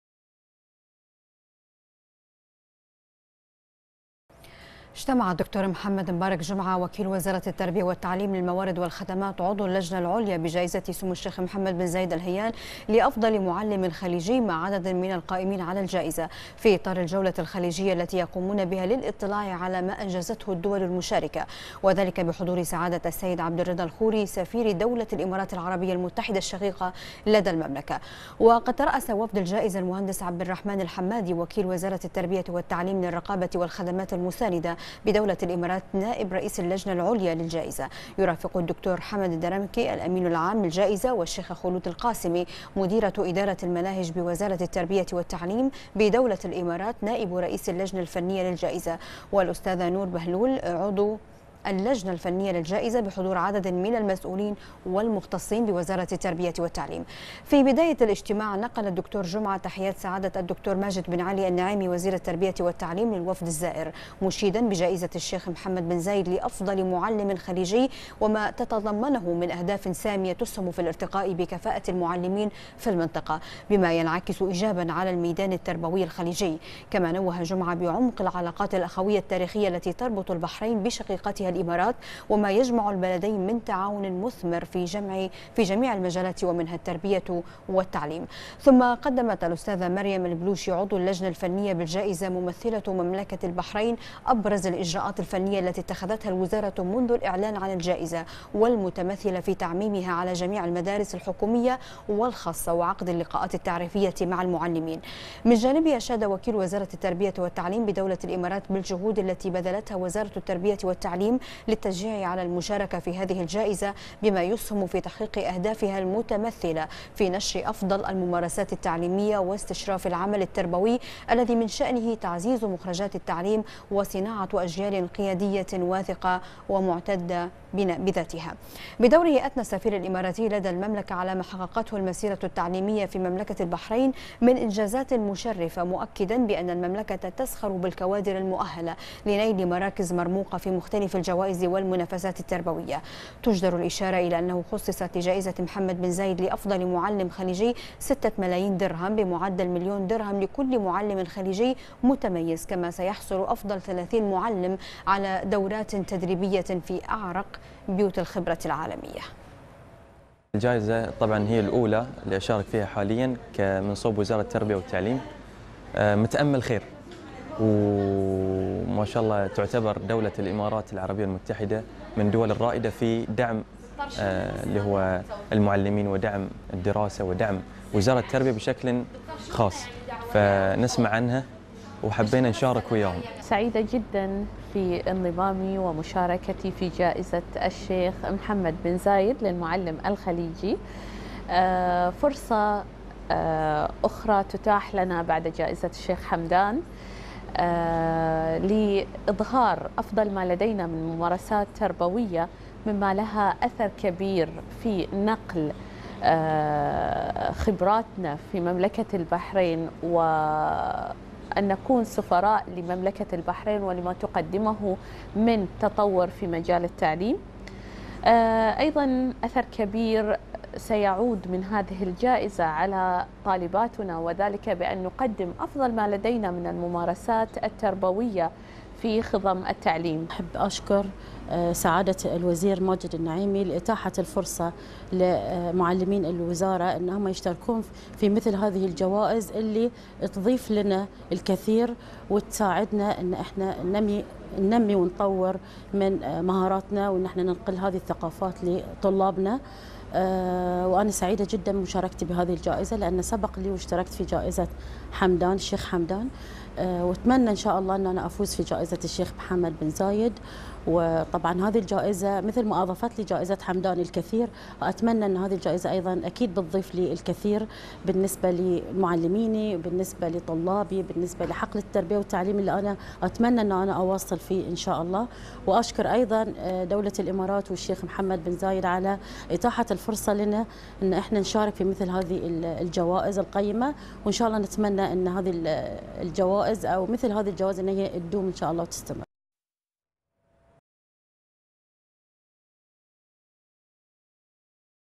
البحرين : وزارة التربية والتعليم تدعو المعلمين للمشاركة في جائزة محمد بن زايد لأفضل معلم خليجي
الوصف

وزارة التربية والتعليم تدعو المعلمين للمشاركة في جائزة محمد بن زايد لأفضل معلم خليجي